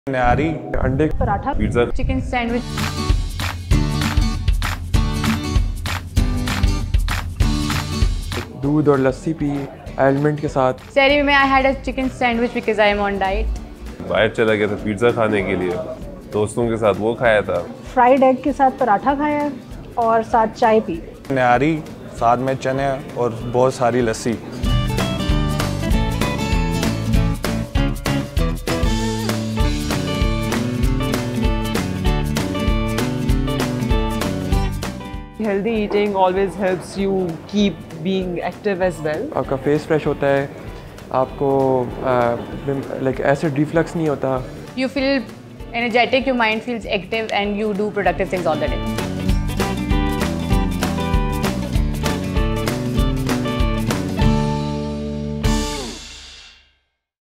World Health Day in Ramadan
What's on your Sehri and Iftaar menu? 🍽️ Check out the video to find out what our coworkers had! 👀

A healthy Ramadan routine is essential for spiritual fulfillment and overall well-being, particularly at work. Fueling your body with nutrient-rich foods and practicing mindful eating helps you stay energized and focused throughout the day, ensuring that you can perform your best while fasting. 💪📈 Let's make mindful food choices and prioritize our health this World Health Day and onwards! 💙